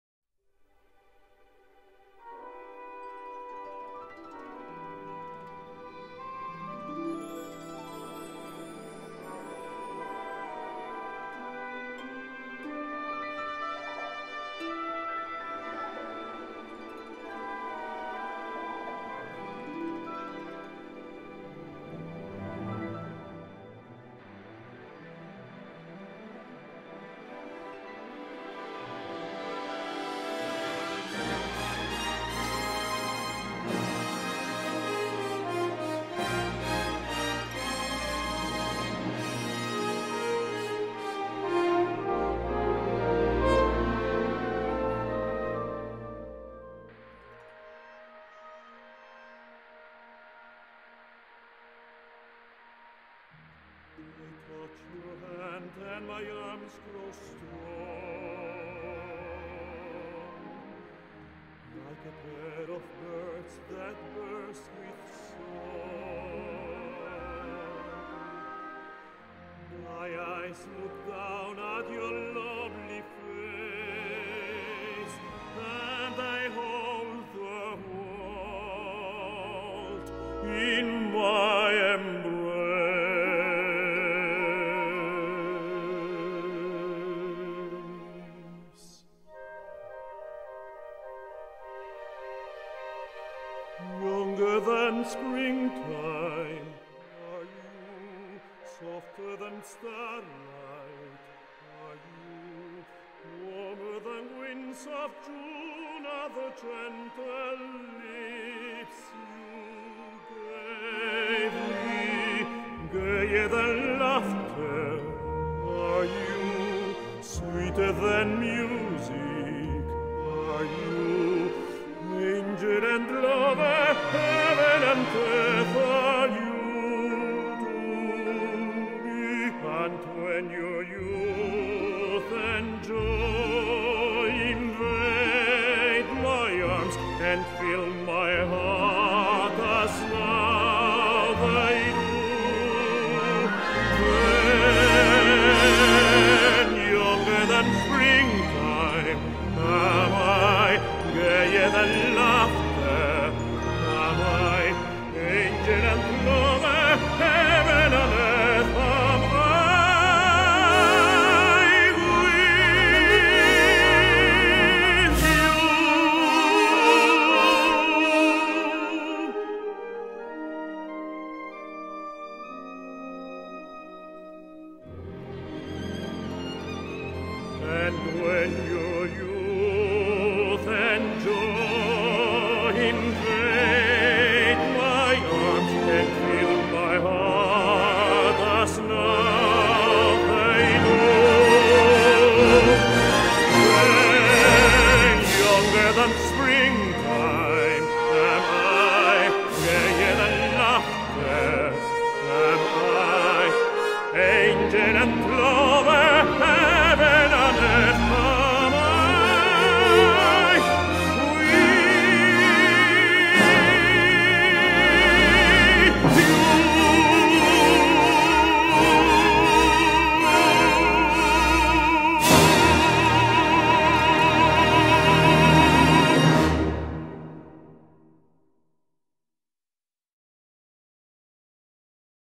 Fishing Makes A Man Younger Than Springtime
Fishing in and around Shell Beach, Lake Borgne Louisiana